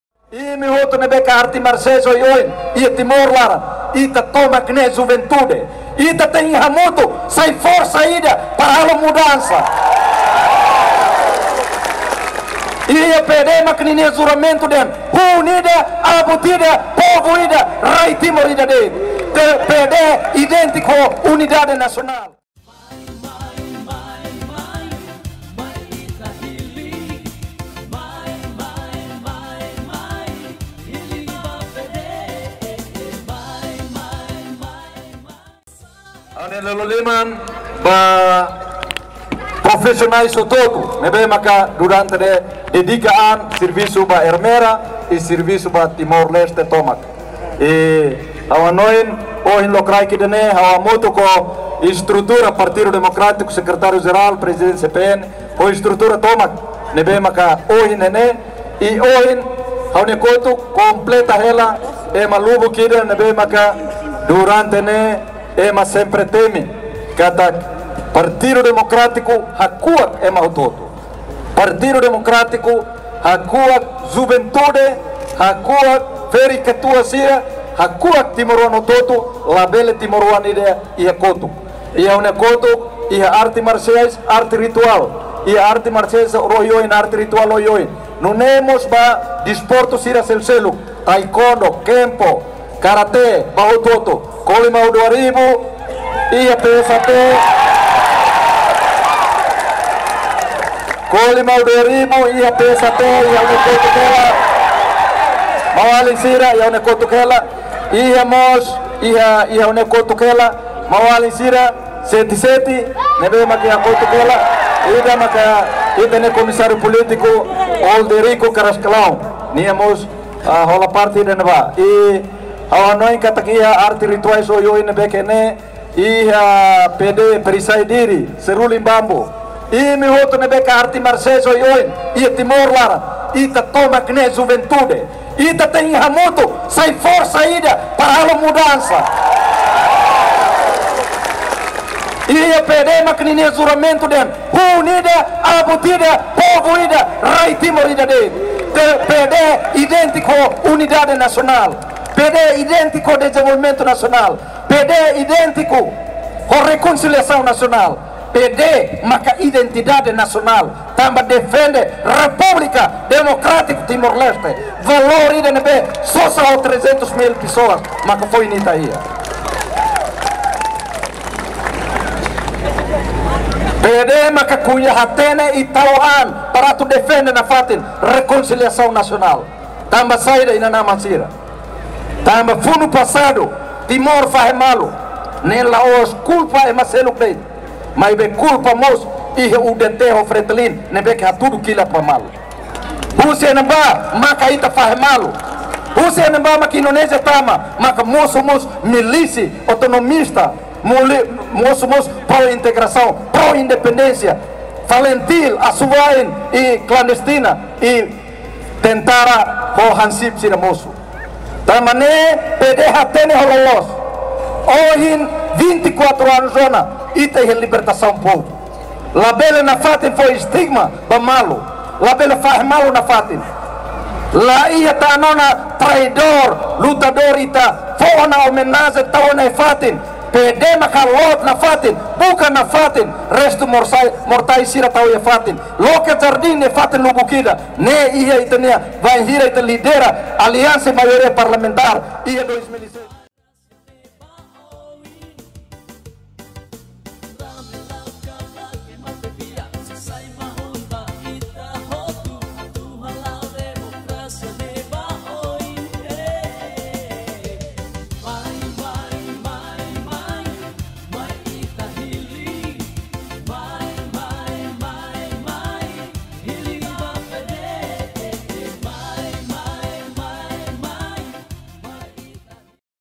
Assanami : Ita Mai Husi Hun ida Abut Ida Povu Ida Rai Timor Ida
Assanami Apela Ba Organizasaun Arte Marsiais no Arte Rituais Oioin atu Hamutuk Sai Forsa ida Hodi Halo Mudansa ba Ita Nia Rain